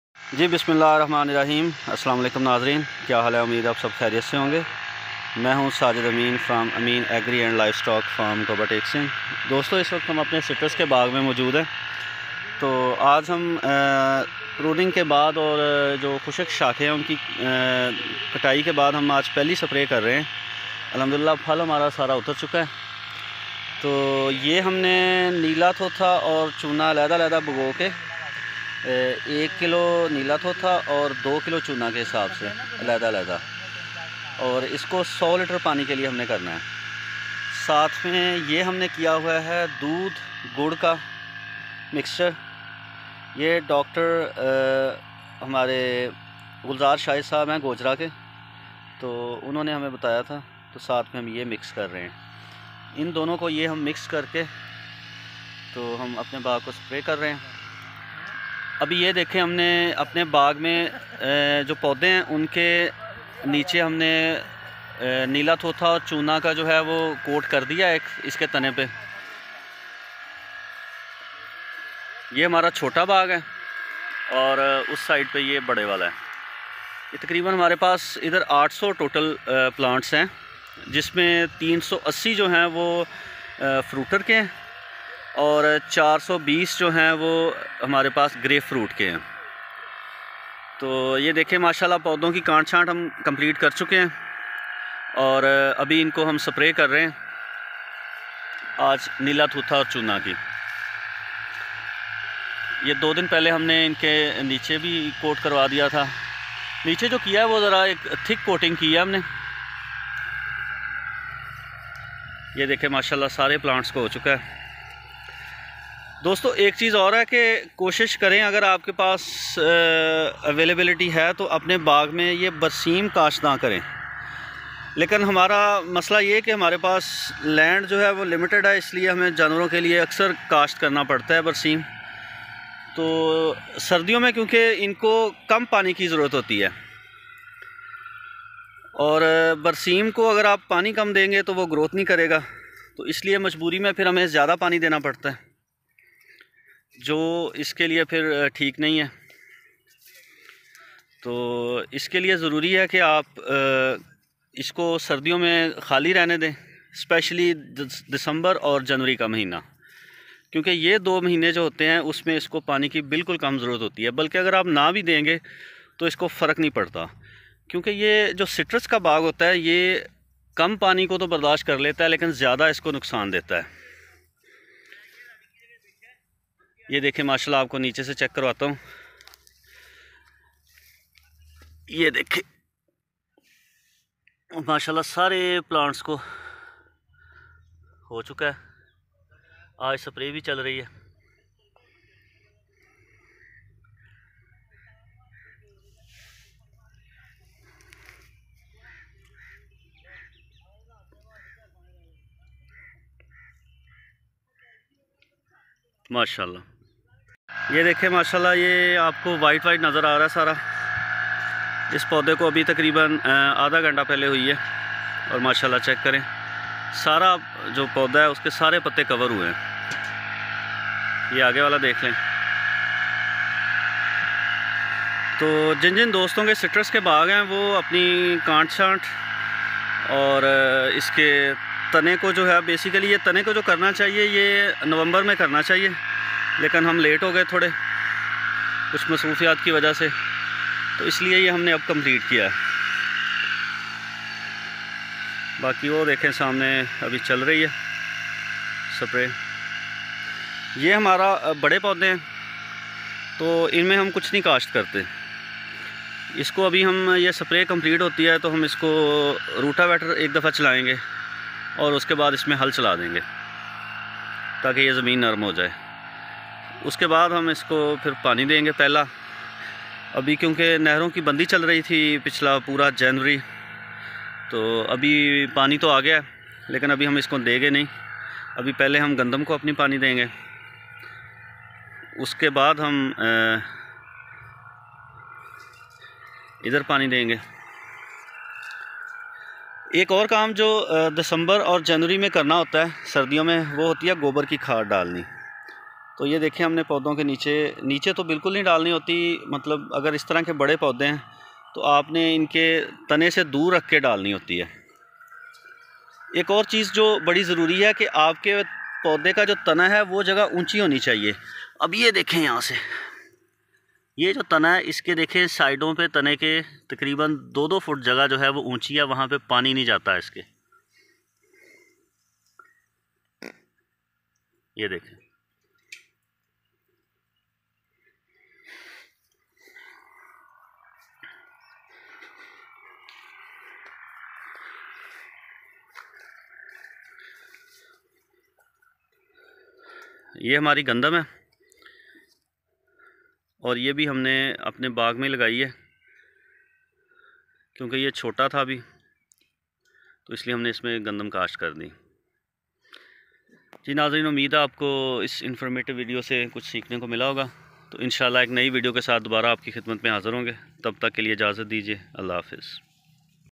Citrus orchards Management in January & February | Bordo paste application | Pruning
In this video we are going to tell you about our practical experience of citrus orchard farming. Bordo paste application on trunks and spray is very beneficial to avoid fungal diseases. Pruning and dry branches cutting help healthy branches to grow more.

Or visit our farm in Toba tek Singh, punjab, Pakistan.